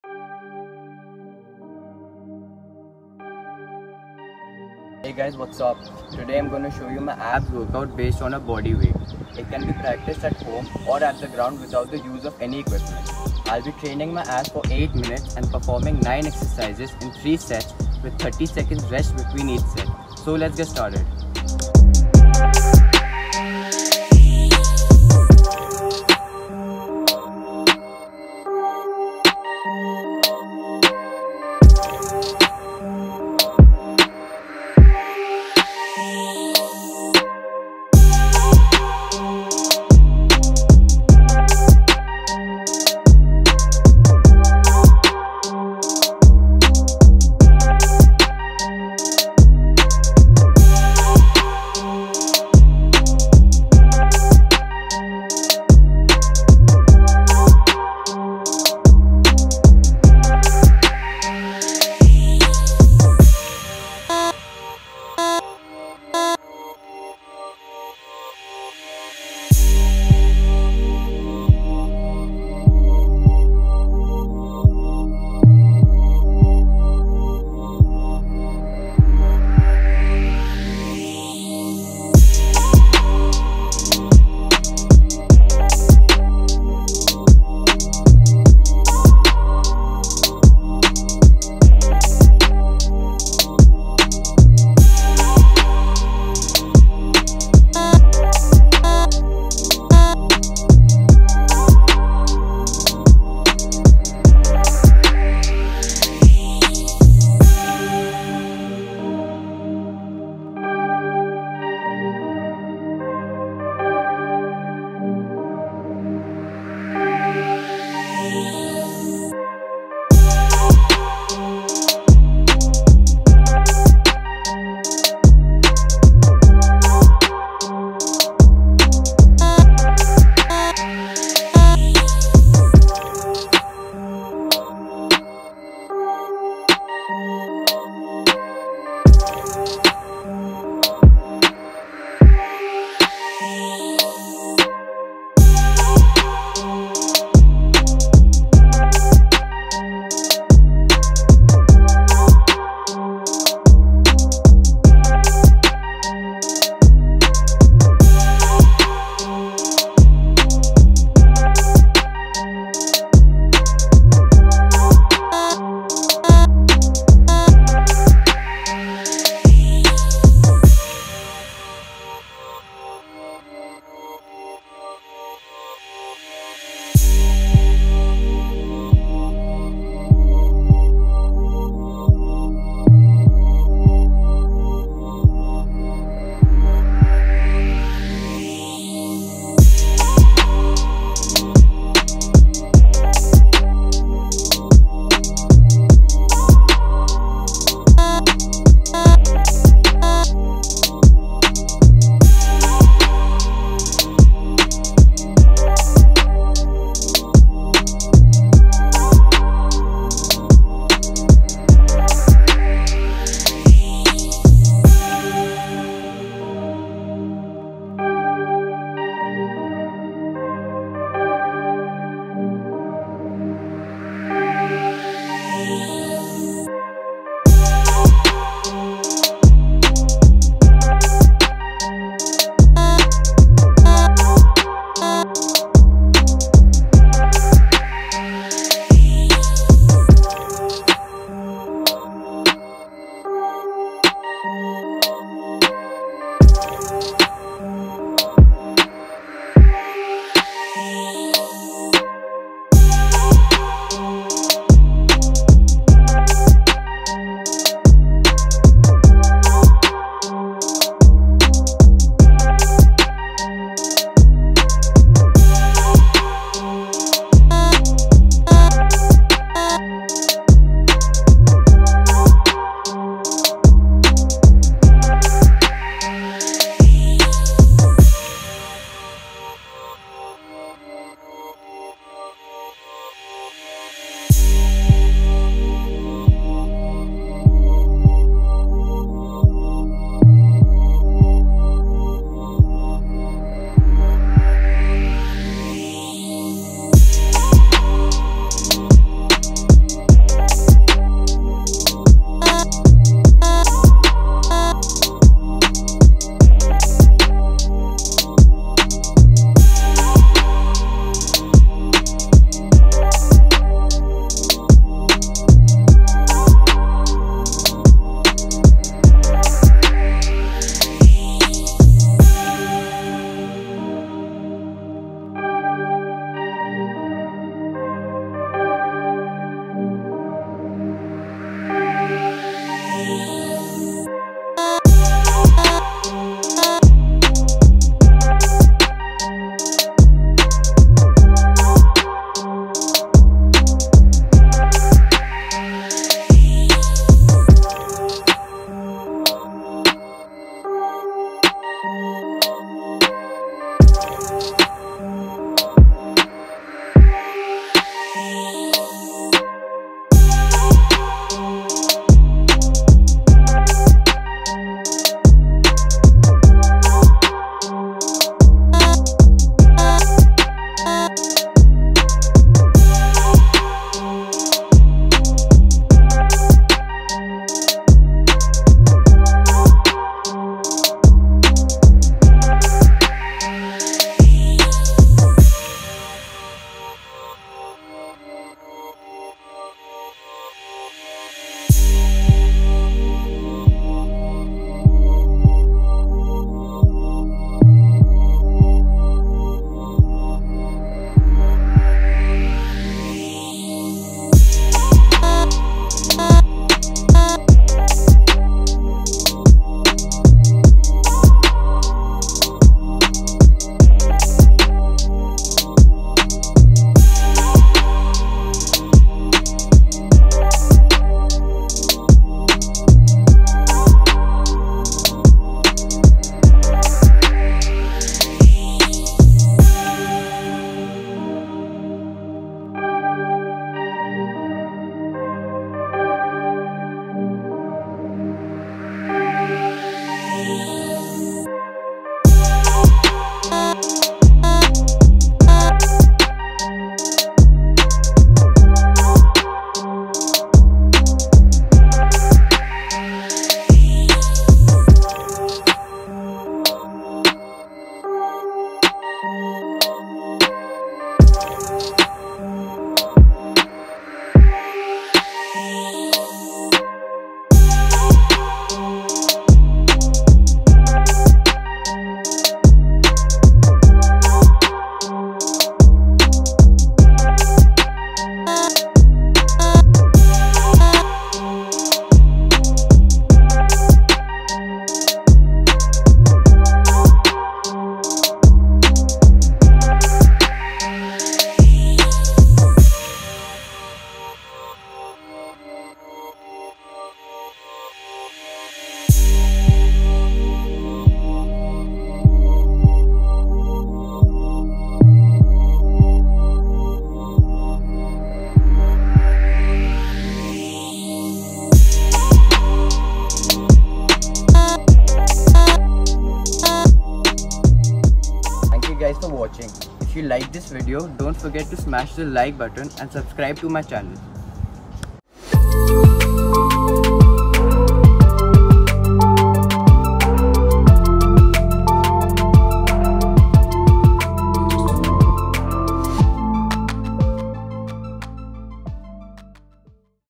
6 PACK ABS WORKOUT FOR BEGINNERS | ABS WORKOUT CHALLENGE | GET ABS IN 2 WEEKS | INDIAN EDITION
Everyone wants to know the secret to get abs within a short period of time. This workout routine combined with a good diet will get you close to your goal in just a matter of weeks. This workout is great for both beginners and for those who have been training regularly.

Exercises:
1. Scissor leg raises (30 seconds)
2. Leg raises (30 seconds)
3. Flutter kicks (30 seconds)
4. Bicycle crunches (30 seconds)
5. Russian twist (30 seconds)
6. Side plank (30 seconds each side)
7. Chair sit ups (30 seconds)
8. Seated in and out (30 seconds)
9. Plank + Plank knee to elbow (30 seconds each side)

Do 3 sets to complete the workout.